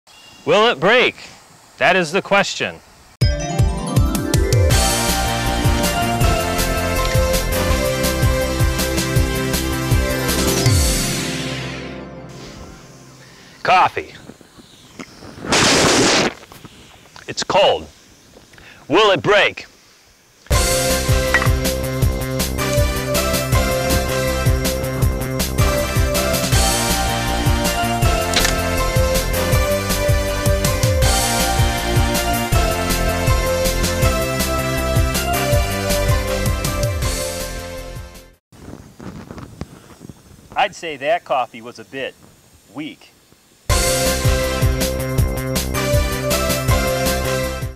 Will it Break? Coffee Mug | G5 Montec
The G5 Montec is put to the ultimate test once again!